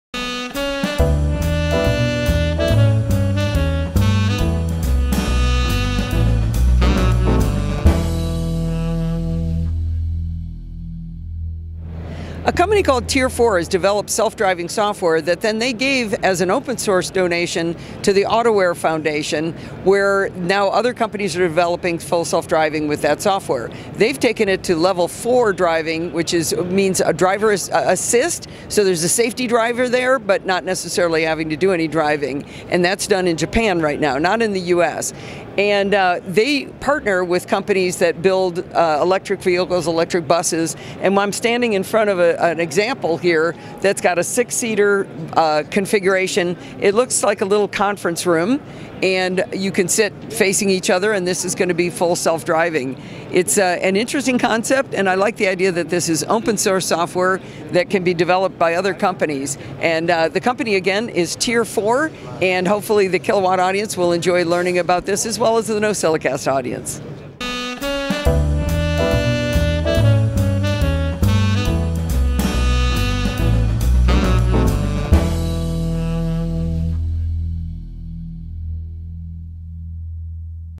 CES 2024: TIER IV Level 4 Self-Driving Software for EVs
Allison Sheridan discusses TIER IV’s efforts and successes in developing autonomous driving (AD) software for electric vehicles. With a strong focus on safety, innovation, and open-source software, TIER IV aims to empower people and revolutionize the way people live and interact with transportation. Based in Japan, TIER IV has successfully achieved Level 4 AD services in select regions in Japan and is working to expand that reach to 50 regions by 2025 and 100 regions by 2027. In parallel, TIER IV is working to expand its AD services to the U.S. To help promote AD across the industry, TIER IV has contributed its AD software and algorithms to the Autoware Foundation, the world’s largest open-source AD project. Allison also shows the Robobus, one of the vehicles TIER IV is integrating its self-driving software and hardware into. Developed by PIX Moving, the Robobus vehicle is an autonomous driving electric shuttle that can seat up to six people comfortably. It features reconfigurable seating, large windows for excellent viewing, and large sliding doors for easy entry and exit. The interview setting is the CES show floor in the Las Vegas Convention Center.